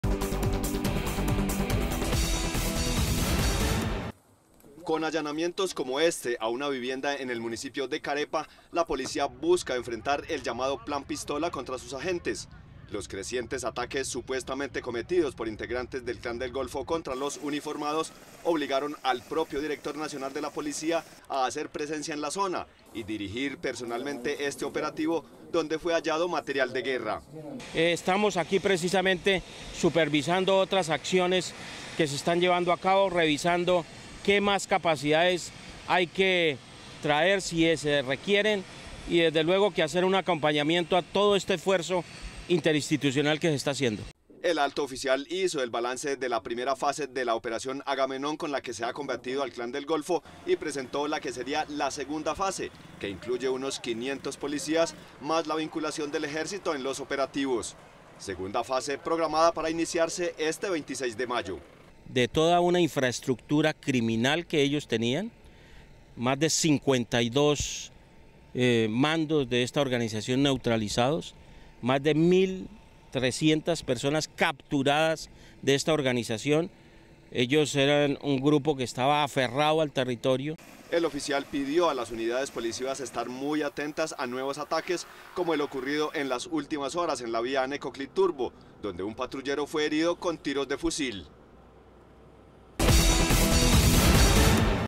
Director general de la Policía dio nuevas instrucciones para enfrentar el “Plan Pistola”
En su visita a la región de Urabá, el general Jorge Hernando Nieto, Director general de la Policía, dio nuevas instrucciones a los uniformados para enfrentar el denominado “Plan Pistola”, liderado por el Clan del Golfo. El alto oficial acompañó a la tropa en varios de los operativos que se realizan en la región.

-Teleantioquia Digital-